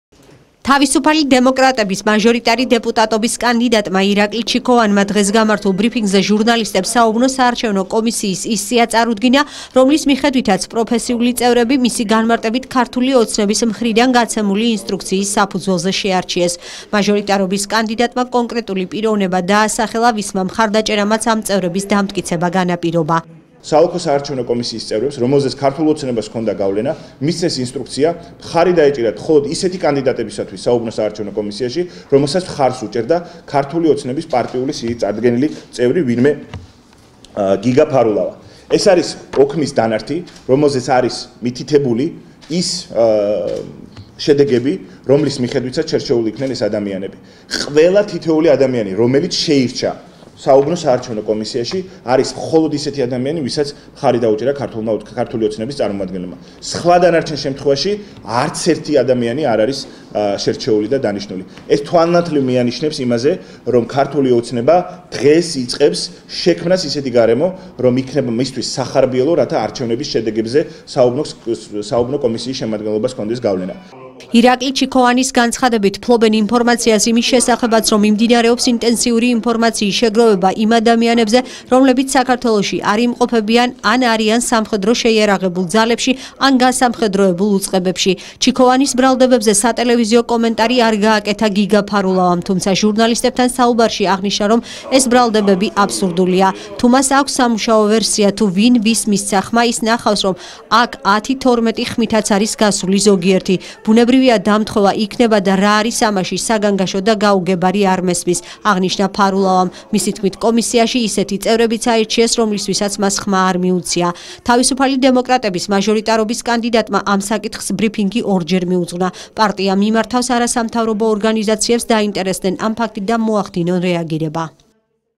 პარტია მიმართავს არასამთავრობო ორგანიზაციებს დაინტერესდნენ ამ ფაქტით და მოახდინონ რეაგირება.
„თავისუფალი დემოკრატების“ მაჟორიტარი დეპუტატობის კანდიდატმა ირაკლი ჩიქოვანმა დღეს გამართულ ბრიფინგზე ჟურნალისტებს საუბნო საარჩვენო კომისიის ის სია წარუდგინა, რომლის მიხედვითაც  პროფესიული წევრები ქართული ოცნების მხრიდან გაცემული ინსტრუქციის საფუძველზე შეარჩიეს- განაცხადა ჩიქოვანმა.მაჟორიტარობის კანდიდატმა კონკრეტული პიროვნება დაასახელა, რომელიც ვისმა მხარდაჭერამაც ამ წევრების დამტკიცება დანაპირობა.ირაკლი ჩიქოვანის განცხადებით, ფლობენ ინფორმაციას იმის შესახებაც, რომ  მიმდინარეობს ინტენსიური ინფორმაციის შეგროვება იმ ადამიანებზე, რომლებიც საქართველოში არ იმყოფებიან, ვინც ასევე არის სამხედრო შეიარაღებულ ძალებში, ან გასამხედროებულ უწყებებში.ჩიქოვანის ბრალდებებზე სატელევიზიო კომენტარი არ გააკეთა გიგა ფარულავამ, თუმცა ჟურნალისტებთან საუბარში აღნიშნა, რომ ეს ბრალდებები აბსურდულია.დაახლოებით 700-ზე მეტი განაცხადი იყო შემოსული, 500-ზე მეტი უნდა გასულიყო. ბუნებრივია ხმათა უმრავლესობით გადის. თუ მას აქვს სამუშაო ვერსია, თუ ვინ ვის მისცა ხმა, ის ნახავს, რომ რომ აქ 10- 12 ხმითაც არის გასული ზოგიერთი. ბუნებრივია დამთხვევა იქნება და რა არის ამაში საგანგაშო და გაუგებარი არ მესმის”- აღნიშნა ფარულავამ.მისი თქმით, კომისიაში ისეთი წევრებიც აირჩიეს, რომლისთვისაც მას ხმა არ მიუცია.„თავისუფალი დემოკრატების“ მაჟორიტარობის კანდიდატმა ამ საკითხს ბრიფინგი ორჯერ მიუძღვნა.პარტია მიმართავს  არასამთავრობო ორგანიზაციებს დაინტერესდნენ ამ ფაქტით და მოახდინონ რეაგირება.